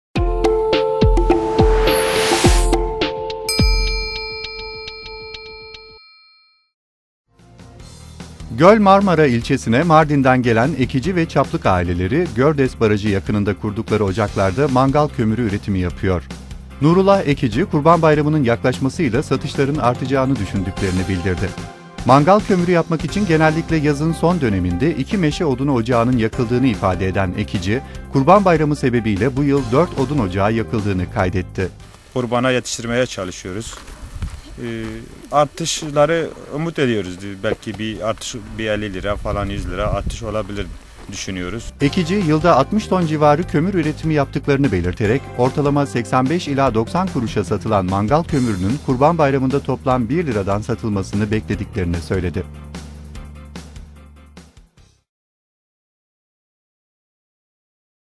Mangal kömürü üreticilerinin umudu kurban bayramı
Mangal Kömürü Üreticileri umutla Kurban Bayramını bekliyor. Üreticiler, artan taleple birlikte ortalama 85-90 kuruşa satılan mangal kömürünün Kurban Bayramında toptan 1 liradan satılmasını umut ediyor.